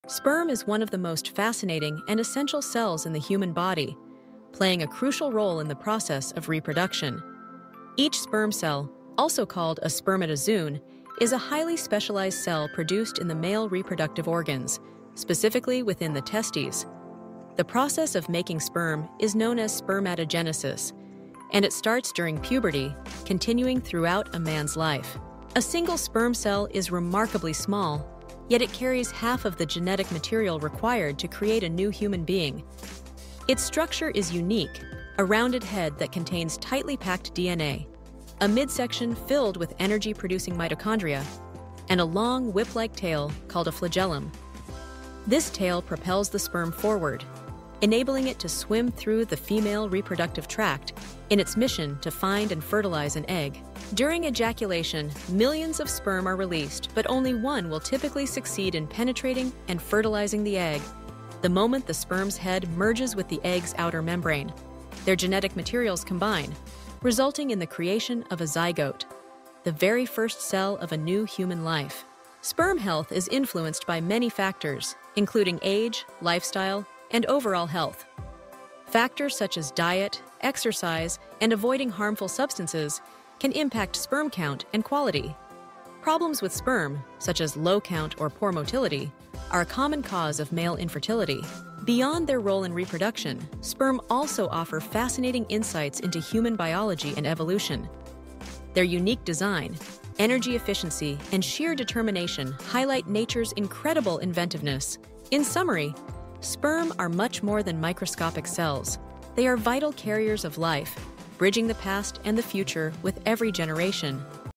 What Is Sperm?
Sperm are tiny but powerful cells that carry the genetic code needed to create life. In this video, discover what sperm really are and why they’re so important for reproduction.